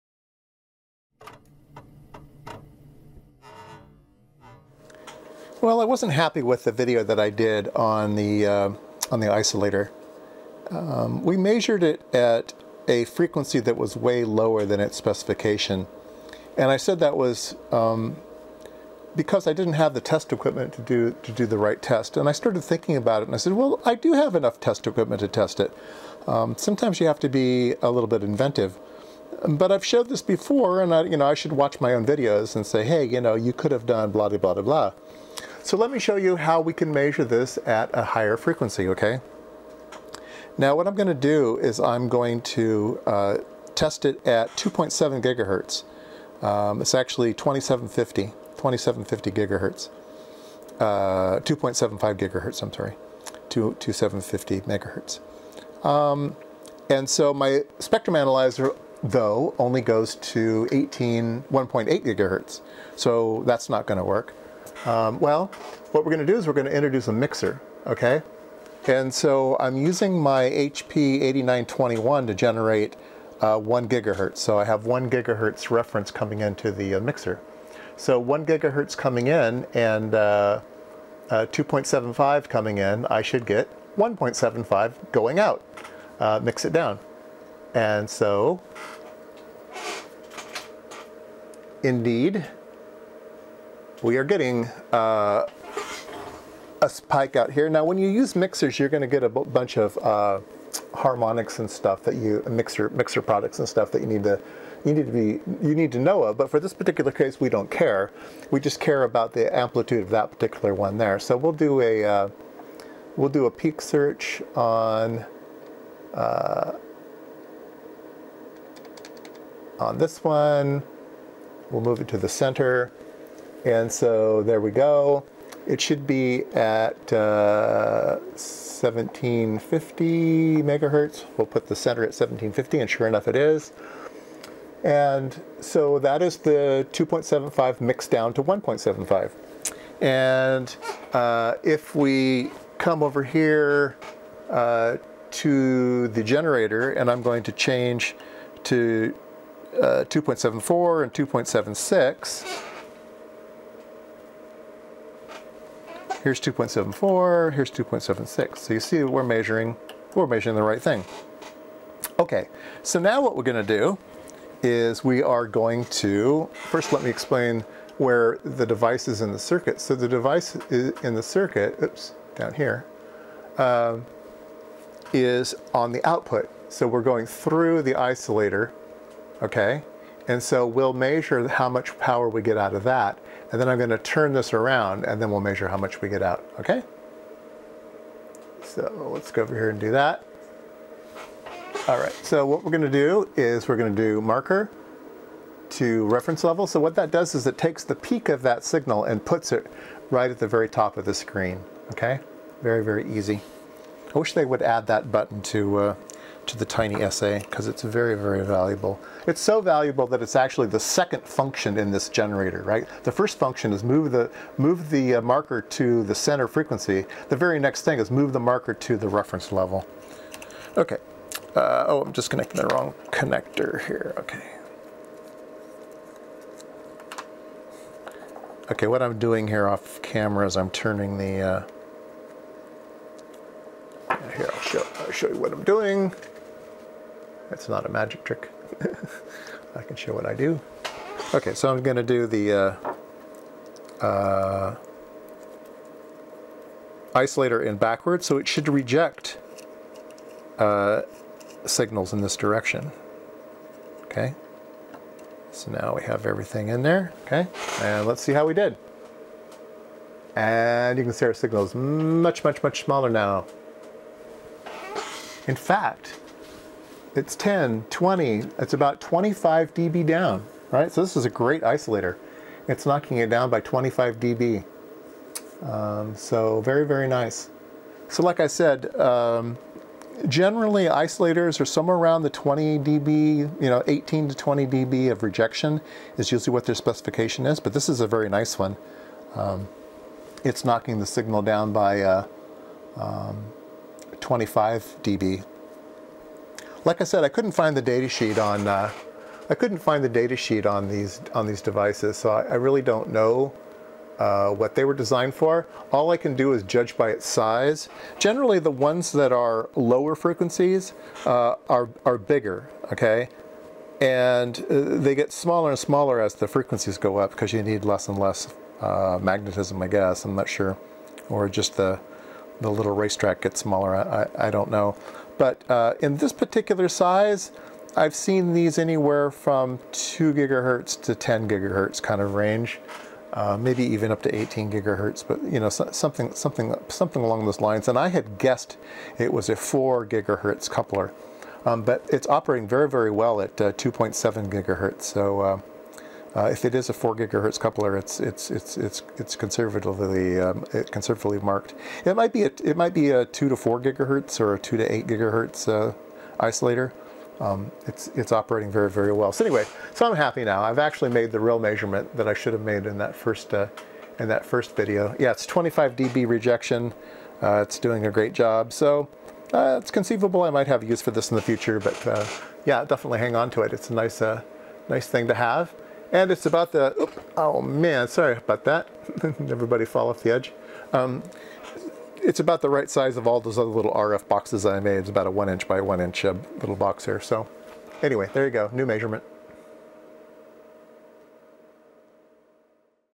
#792 RF Isolator Circulator (part 2 of 2)
Episode 792
A circulator with a 50 ohm load on one port is called an isolator. Let's see how they work and measure one.